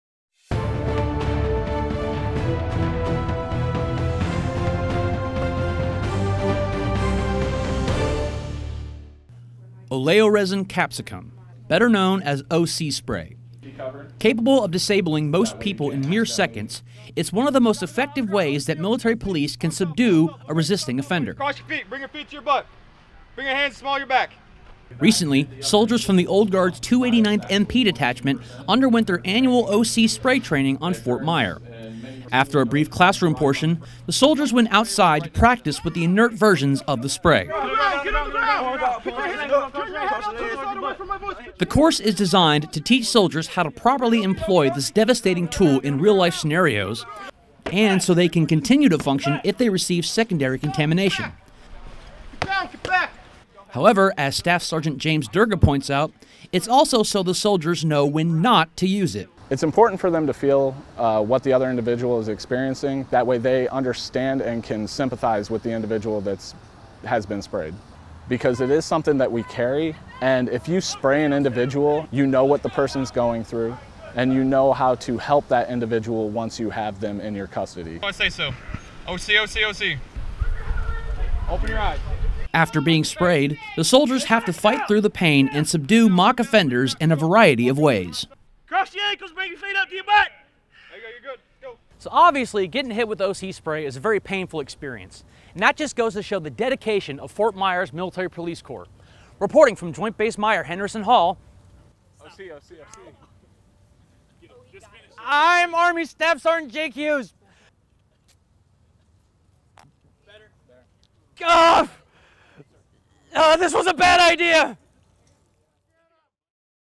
289th MP Company OC Spray Training
Soldiers from The Old Guard's 289th Military Police Company recently underwent training with OC Spray. More commonly known as "Pepper Spray," it is one of the most effective tools in the MP's arsenal. (U.S. Army video by SSG Jake Hughes)